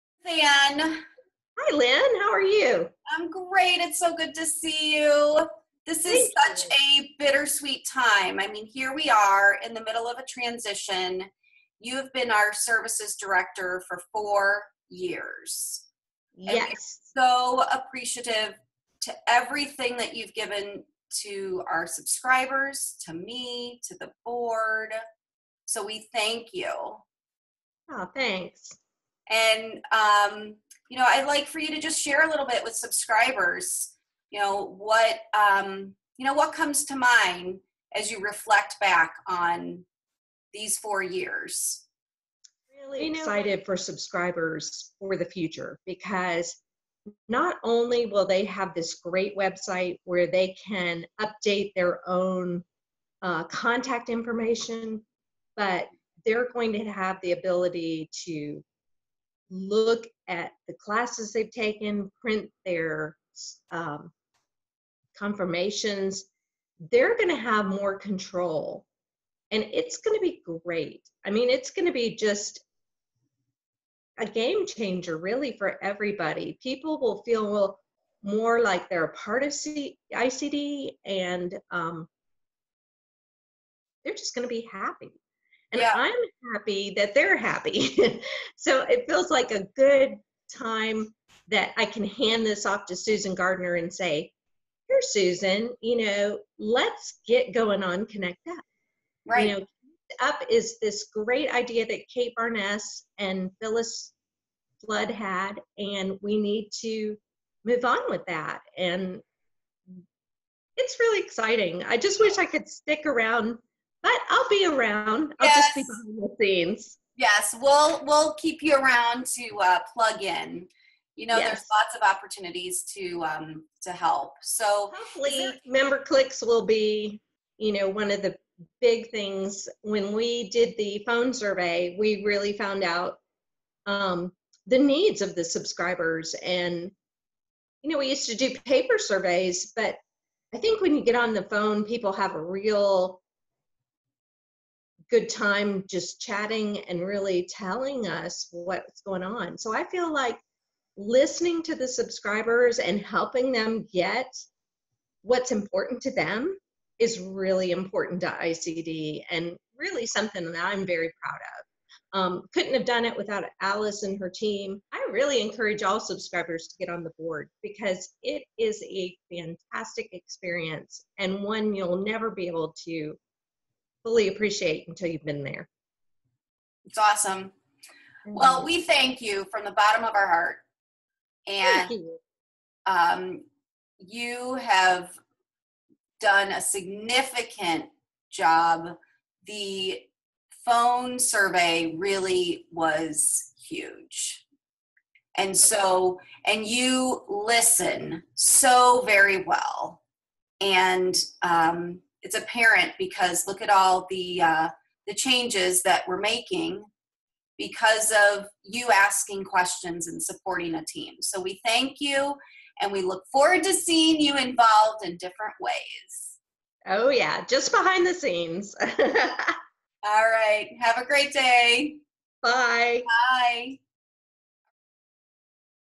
Thank you Ruthann
Thank you to Ruthann for serving as the Services Director for ICD from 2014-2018.